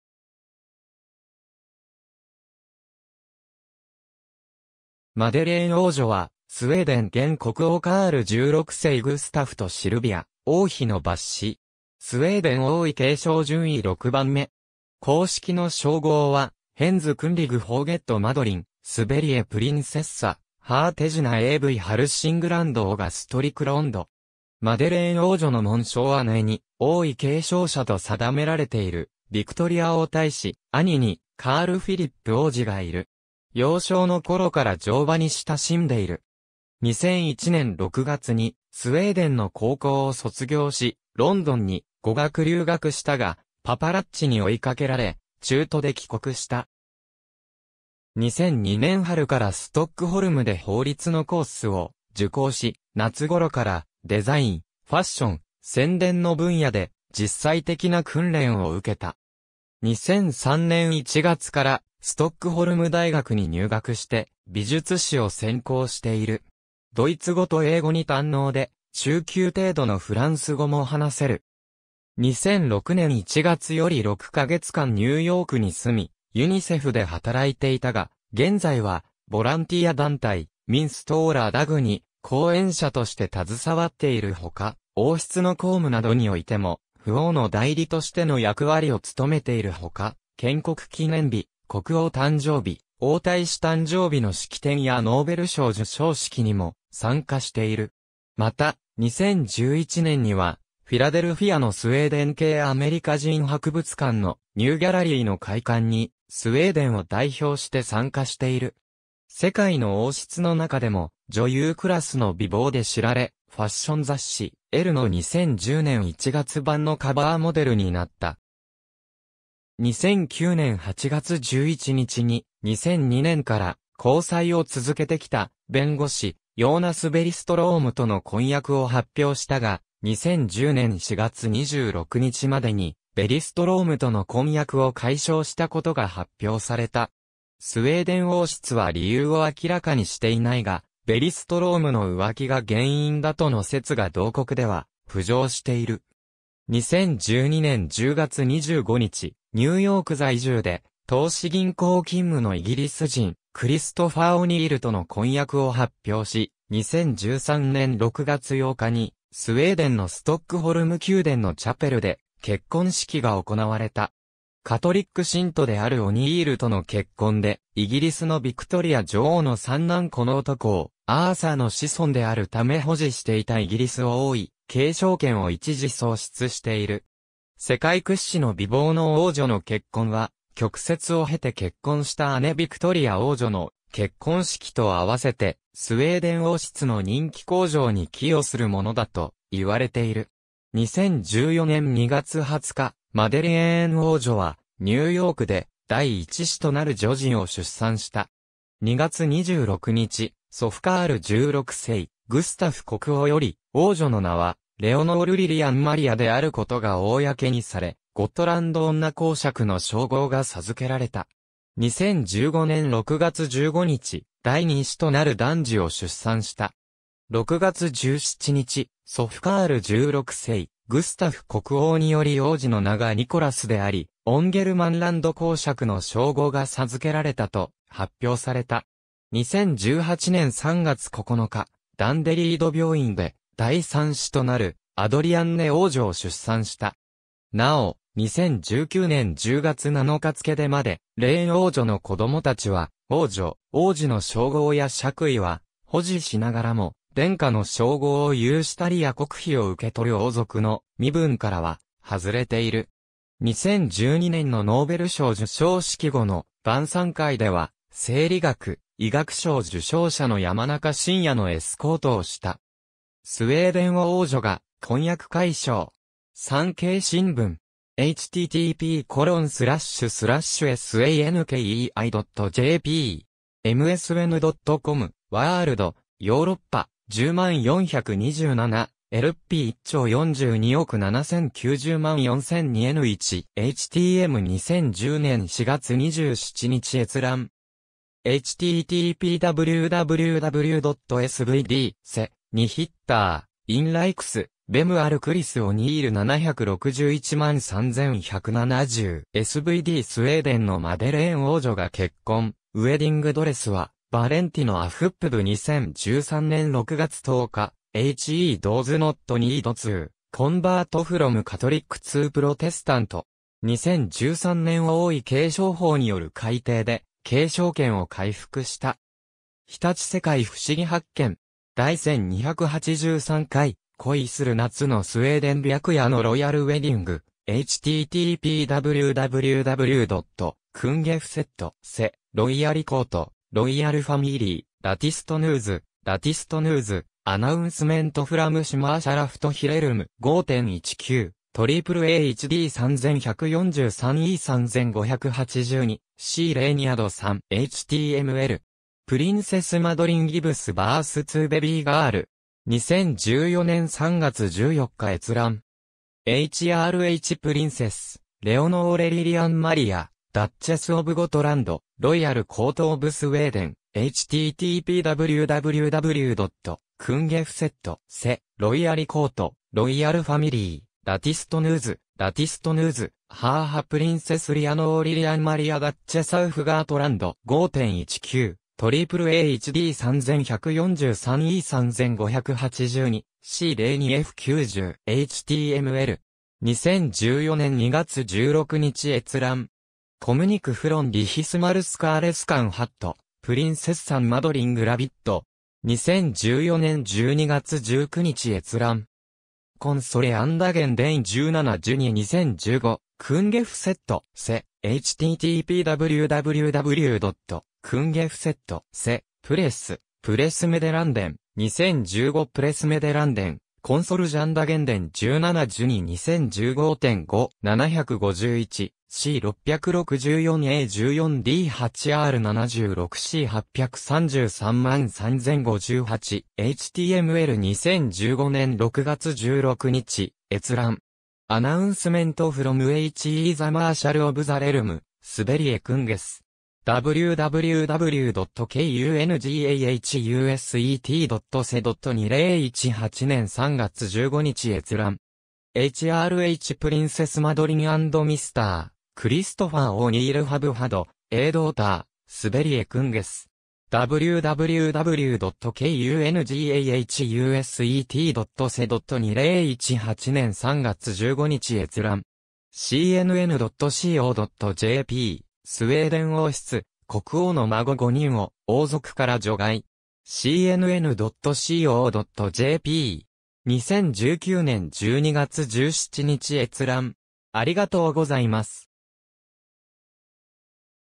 マデレーン (スウェーデン王女)
マデレーン王女（Prinsessan Madeleine, Madeleine Thérèse Amelie Josephine Bernadotte, 1982年6月10日 - ）は、スウェーデン（ベルナドッテ王朝）現国王カール16世グスタフとシルヴィア王妃の末子。
スウェーデン王位継承順位6番目。
公式の称号は Hennes Kunglig Höghet Madeleine, Sveriges Prinsessa, Hertiginna av Hälsingland och Gästrikland（ヘルシングランドおよびイェストリークランド公爵マデレーン王女殿下）。
マデレーン王女の紋章 姉に王位継承者と定められているヴィクトリア王太子、兄にカール・フィリップ王子がいる。
幼少の頃から乗馬に親しんでいる。
2001年6月にスウェーデンの高校を卒業し、ロンドンに語学留学したが、パパラッチに追いかけられ、中途で帰国した。
2002年春からストックホルムで法律のコースを受講し、夏ごろからデザイン、ファッション、宣伝の分野で実際的な訓練を受けた。
2003年1月からストックホルム大学に入学して美術史を専攻している。
ドイツ語と英語に堪能で、中級程度のフランス語も話せる。
2006年1月より6か月間ニューヨークに住み、ユニセフで働いていたが、現在はボランティア団体“Min Stora Dag”（アメリカの“Make a Wish”に相当）に後援者として携わっているほか、王室の公務などにおいても、父王の代理としての役割を務めているほか、建国記念日、国王誕生日、王太子誕生日の式典やノーベル賞授賞式にも参加している。
また、2011年にはフィラデルフィアのスウェーデン系アメリカ人博物館のニューギャラリーの開館にスウェーデンを代表して参加している。
世界の王室のなかでも女優クラスの美貌で知られ、ファッション雑誌『ELLE』（スウェーデンエディション）の2010年1月版のカヴァーモデルになった。
2009年8月11日に、2002年から交際を続けてきた弁護士・ヨーナス・ベリストロームとの婚約を発表したが、2010年4月26日までに、ベリストロームとの婚約を解消したことが発表された。
スウェーデン王室は理由を明らかにしていないが、ベリストロームの浮気が原因だとの説が同国では浮上している。
2012年10月25日、ニューヨーク在住で投資銀行勤務のイギリス人クリストファー・オニールとの婚約を発表し、2013年6月8日にスウェーデンのストックホルム宮殿のチャペルで結婚式が行われた。
カトリック信徒であるオニールとの結婚で、イギリスのヴィクトリア女王の三男コノート公アーサーの子孫であるため保持していたイギリス王位継承権を一時喪失している。
世界屈指の美貌の王女の結婚は、曲折を経て結婚した姉ヴィクトリア王女の結婚式と合わせて、スウェーデン王室の人気向上に寄与するものだと言われている。
2014年2月20日、マデレーン王女はニューヨークで第1子となる女児を出産した。
2月26日、祖父カール16世グスタフ国王より王女の名はレオノール・リリアン・マリア（Leonore Lilian Maria）であることが公にされ、ゴットランド女公爵（hertiginna av Gotland）の称号が授けられた。
2015年6月15日、第2子となる男児を出産した 。
6月17日、祖父カール16世グスタフ国王により王子の名がニコラス（Nicholas）であり、オンゲルマンランド公爵（hertig av Ångermanland）の称号が授けられたと発表された。
2018年3月9日、ダンデリード病院で第3子となるアドリアンネ王女を出産した。
なお、2019年10月7日付けでマデレーン王女の子供たちは、王女・王子の称号や爵位は保持しながらも、「殿下」の称号を有したりや国費を受け取る王族の身分からは外れている。
^ 2012年のノーベル賞授賞式後の晩餐会では、生理学・医学賞受賞者の山中伸弥のエスコートをした。
^ “スウェーデン王女が婚約解消”. 産経新聞. (2010年4月27日).